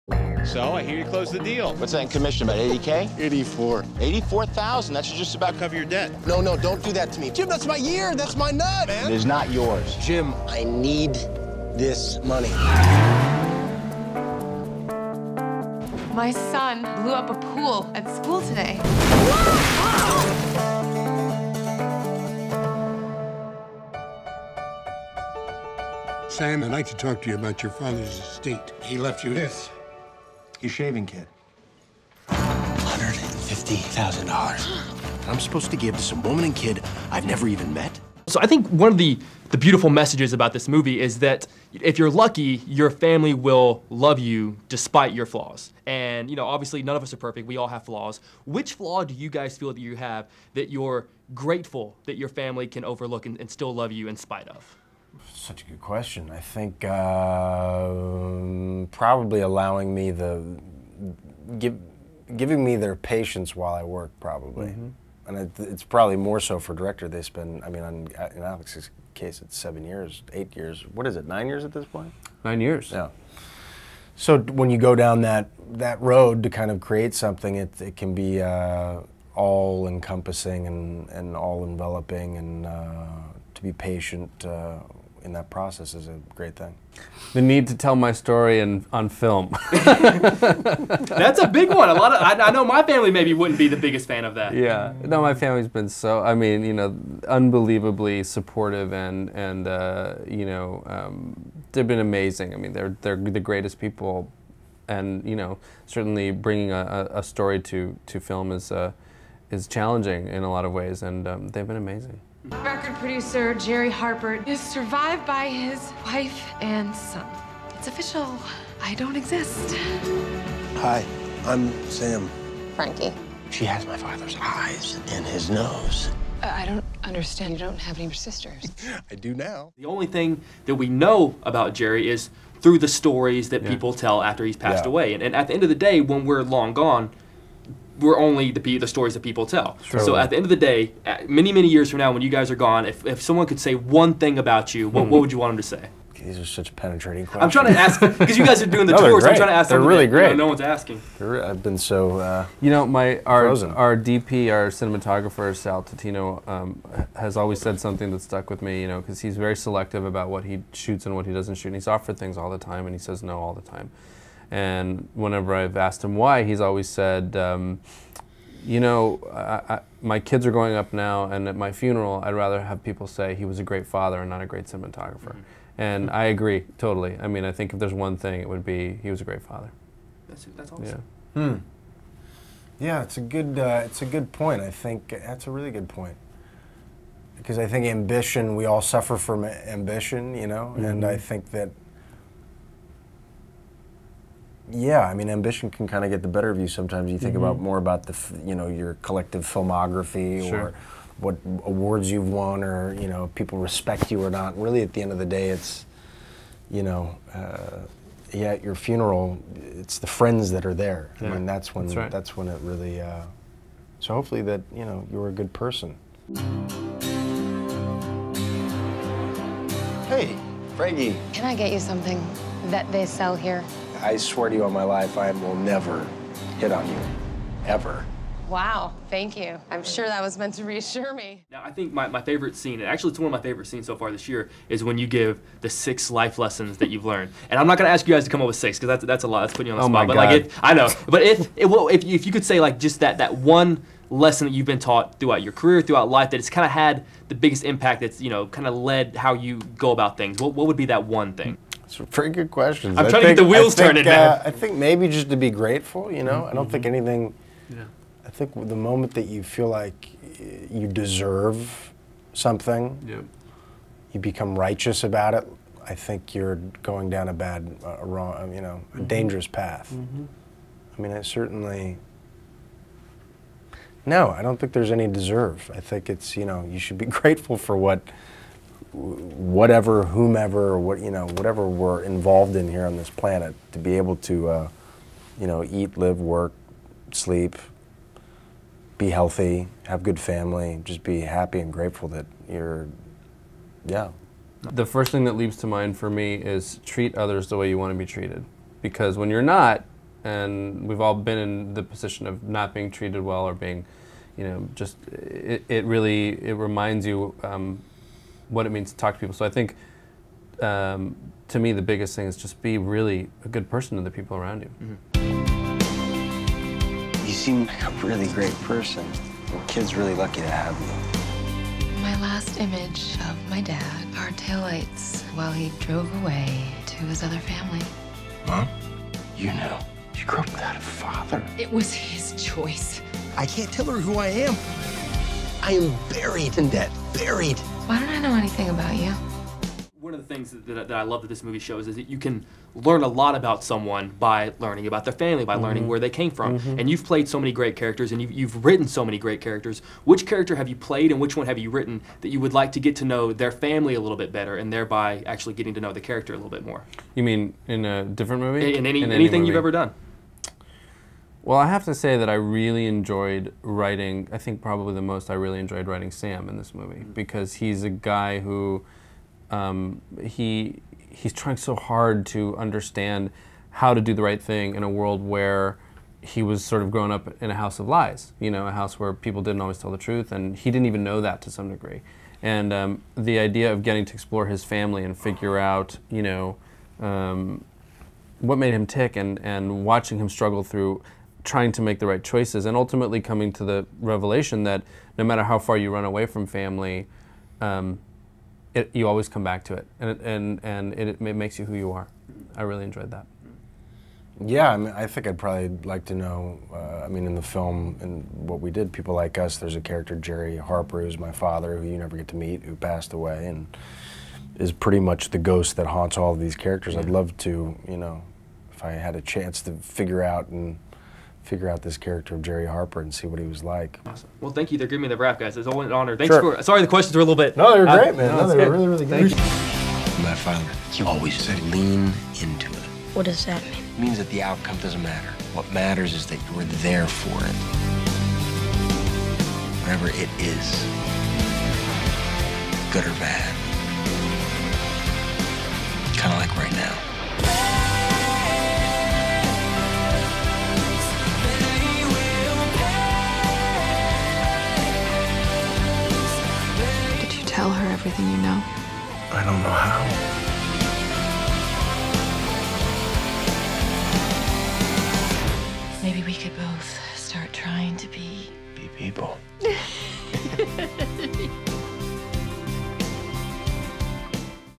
Chris Pine and Alex Kurtzman (Director) Interview for PEOPLE LIKE US
Emmy winner Jake Hamilton sits down to talk with Chris Pine and director Alex Kurtzman about their new film, PEOPLE LIKE US -- only on JAKE'S TAKES!